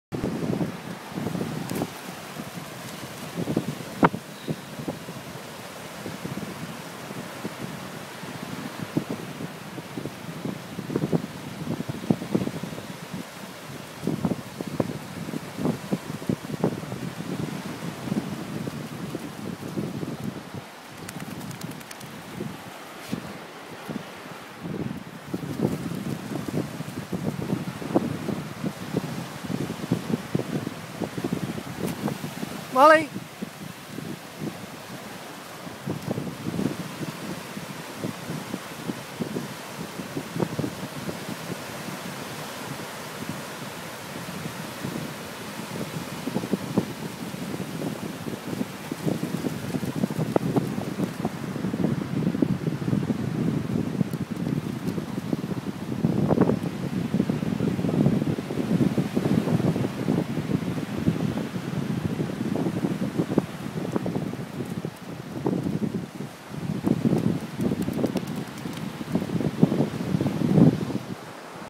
Marley on the beach 5 August
Marley fetching his ball